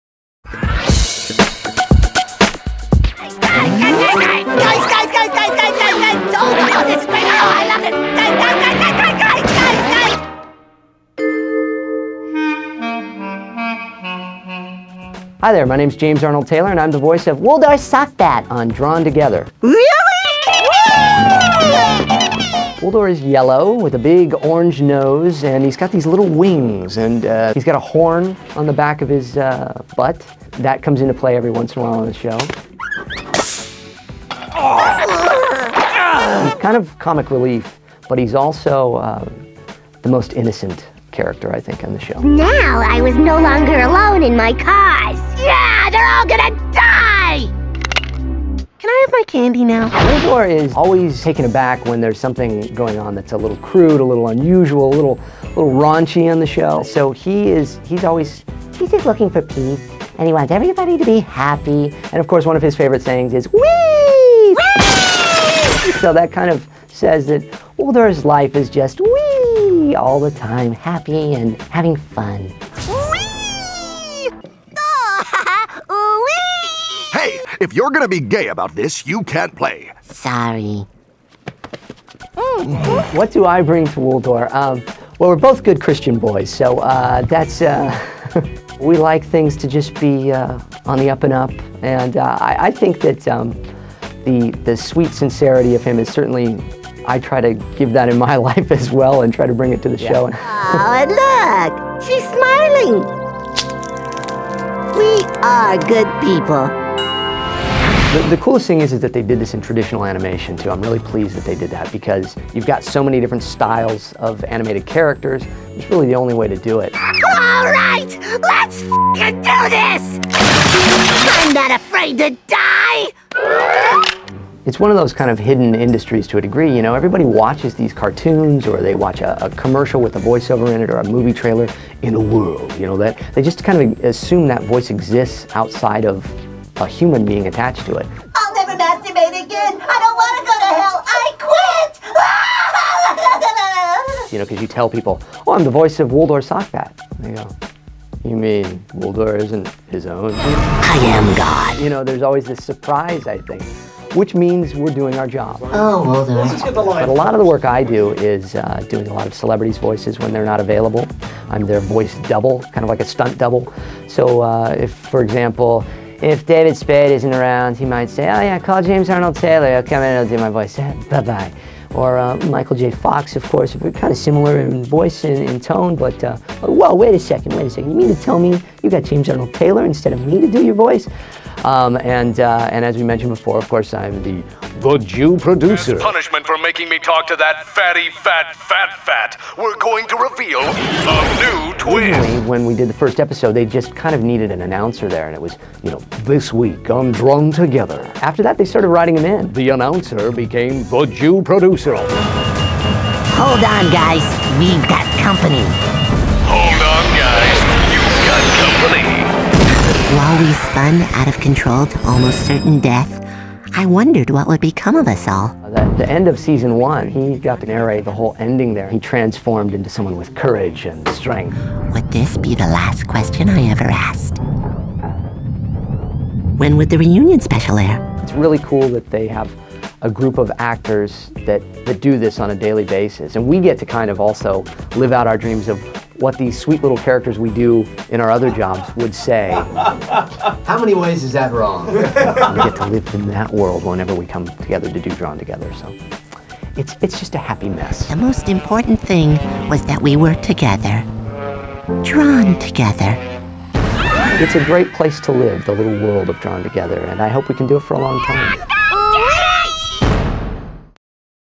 Drawn Together: Voice Actor Interviews - James Arnold Taylor (Wooldoor Sockbat)
Interview with James Arnold Taylor, the voice of Wooldoor Sockbat from Drawn Together.